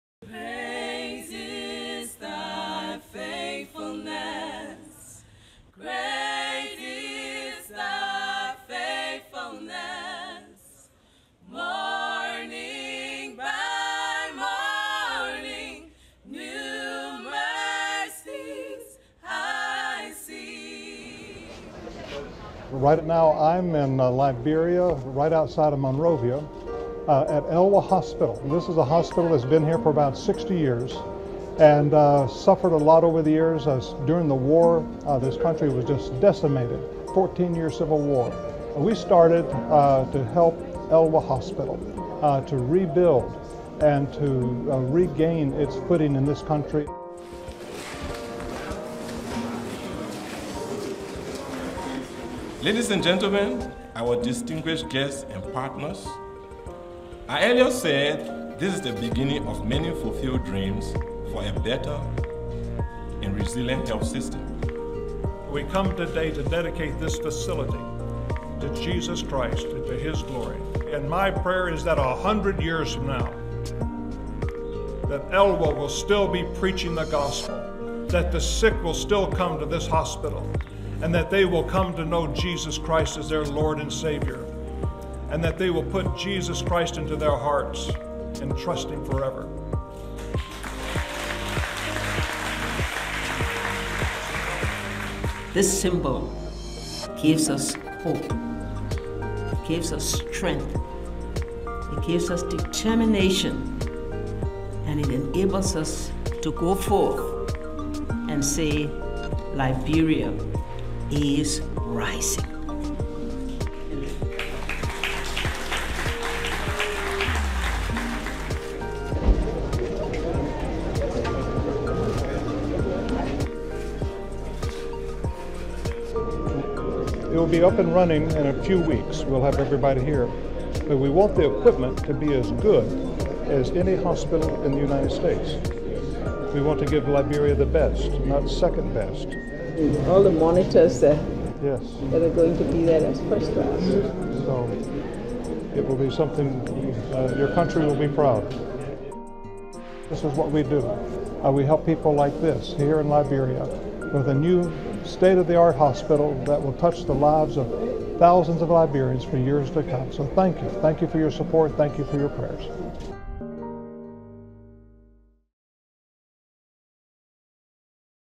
A Gospel Light in Liberia
The newly reconstructed ELWA Hospital in Monrovia, Liberia, is a testimony to God's faithfulness and stands as a beacon of hope for the nation as it struggles to rise after the 2014 Ebola outbreak. Samaritan's Purse President Franklin Graham and the President of Liberia, Ellen Johnson Sirleaf, among other dignitaries, were there recently to dedicate the new facilities to God's glory.